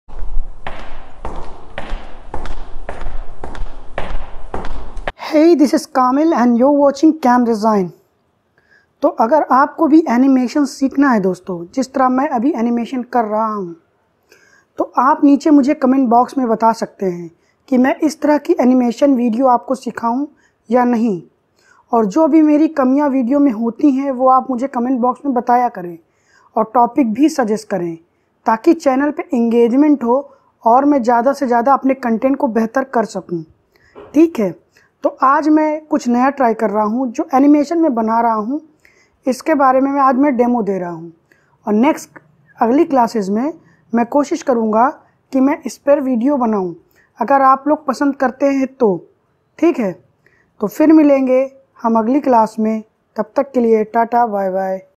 Animation With Help Of VSDC Video Editor And Animaker
This video is combination of two video one is vsdc and other is animaker , I have made animation in animaker and sync audio in vsdc free video editor.